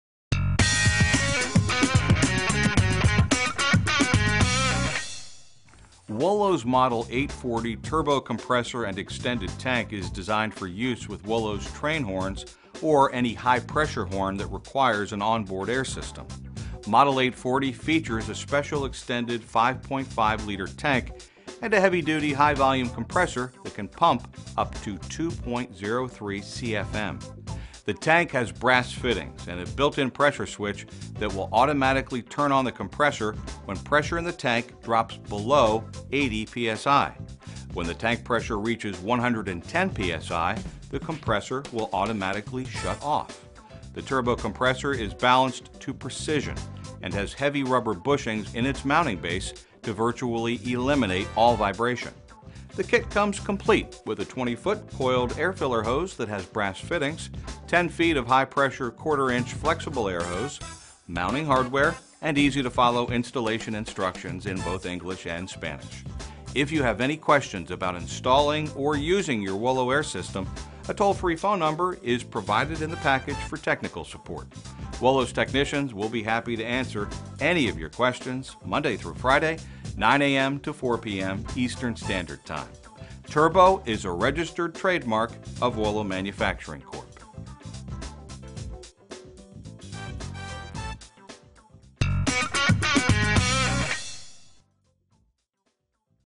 Wolo Turbo Compressor and Tank System for Air Horns - Model# 840
This Wolo Turbo compressor and tank system is designed for use with Siberian Express and Philly Express air horns (Item#s 700528, 700529 and 700530) or any high-pressure horns requiring an onboard air system. It provides special extended 5.5L ...